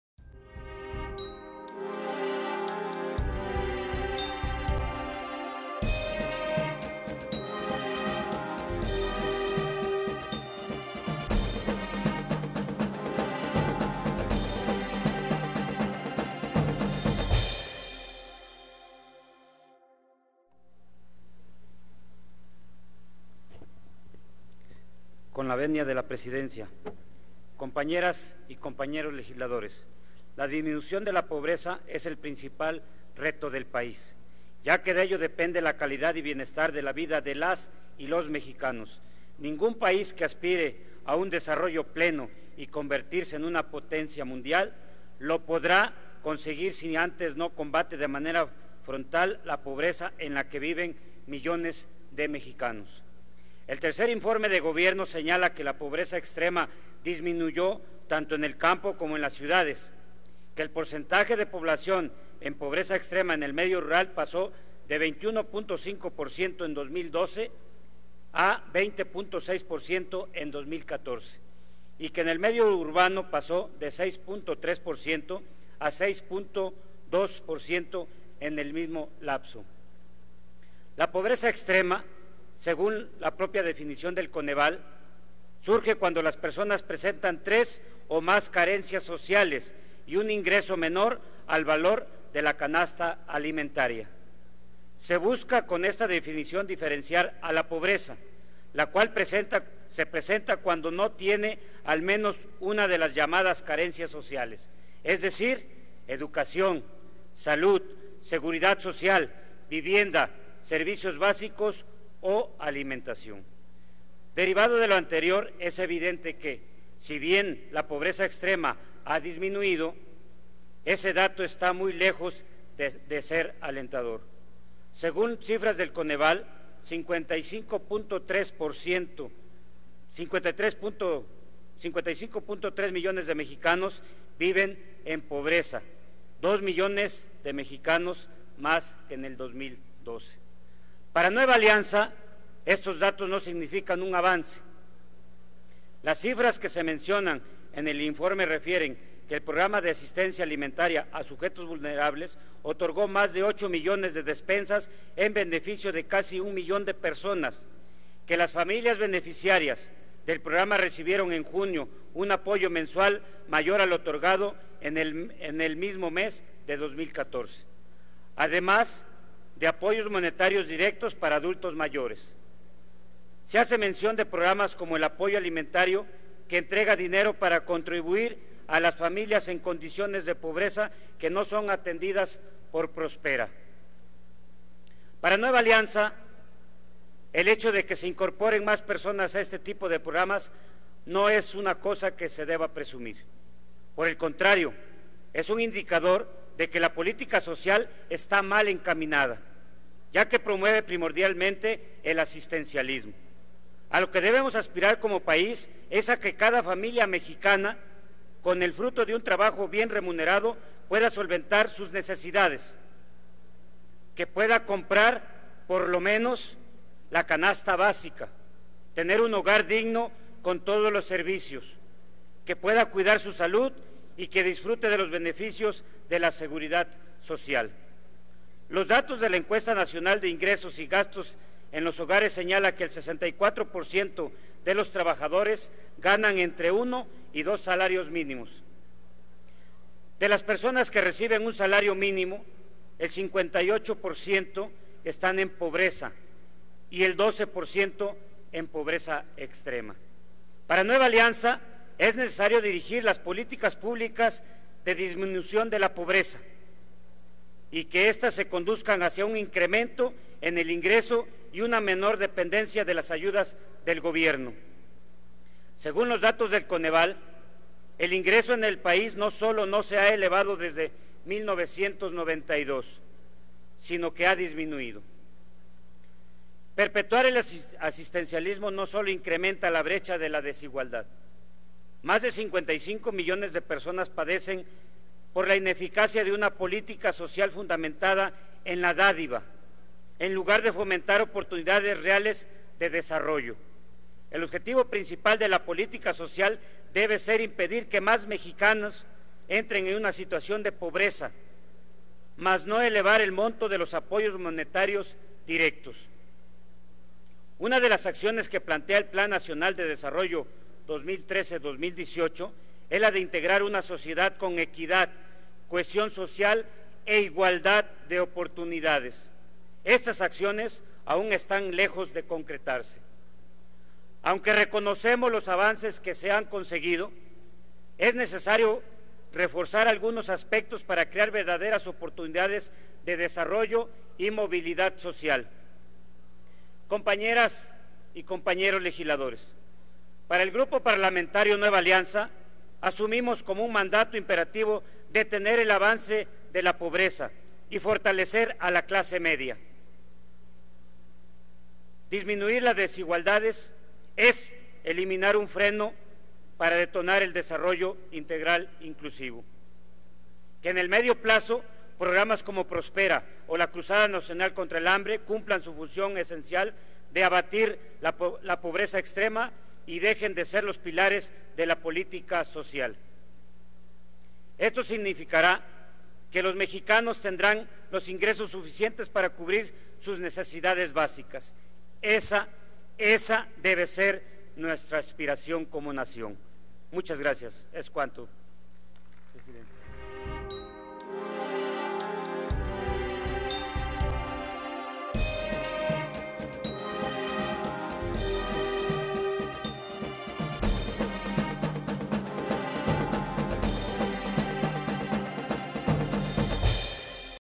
Dip. Ángel García (NA) - Análisis del III Informe (Política Social)
El Dip. Ángel García Yáñez (NA) refiere cifras del Tercer Informe de Gobierno que dan cuenta de la disminución de la pobreza extrema tanto en campo como en las ciudades. Igualmente, señala que de acuerdo con cifras del CONEVAL la pobreza se incrementó en dos millones. Reitera que la política social está mal encaminada y cuestiona la promoción del asistencialismo. Explica que de las personas que reciben un salario mínimo el 58% se encuentra en pobreza y el 12% en pobreza extrema. Finalmente, se pronuncia por detener el avance de la pobreza.